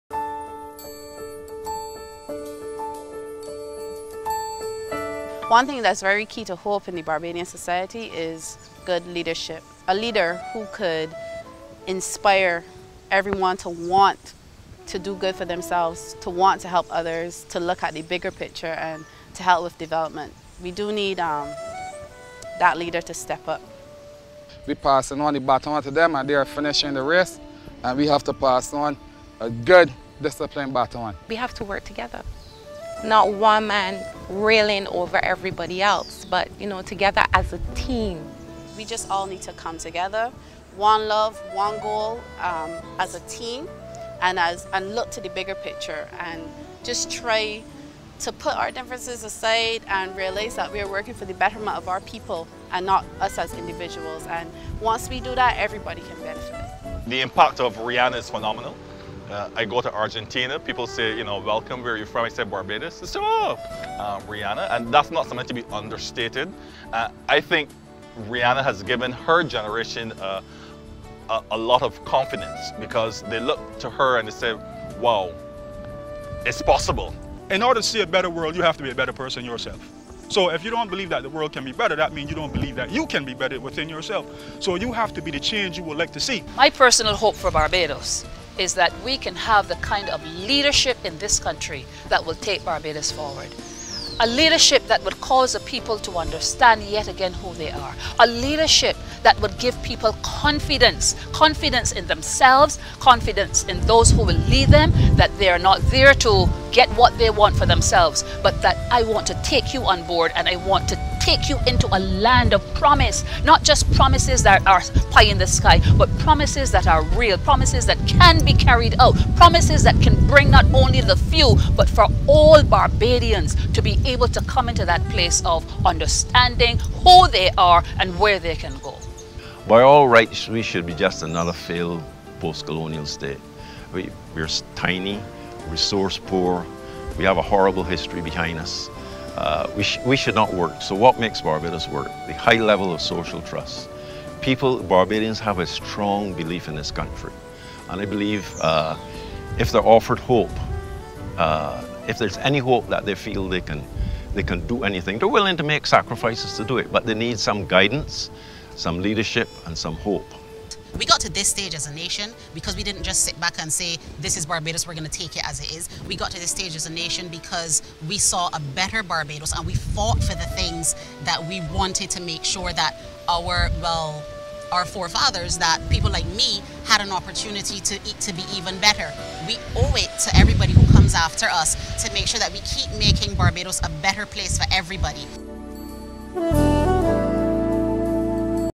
BLP Covenant of Hope - Our Vision for Engaging the World
A Barbadian people fully equipped with the skills, boldness and confidence to compete in the dynamic,international economy of the 21st century, supported by a facilitative domestic and regional policy framework, based firmly on sovereignty, independent and principled positions executed by an enhanced, professionalised, refocused diplomatic and trade representation fully capable of defending our vital national and regional interests in all traditional and emerging centres of power and influence.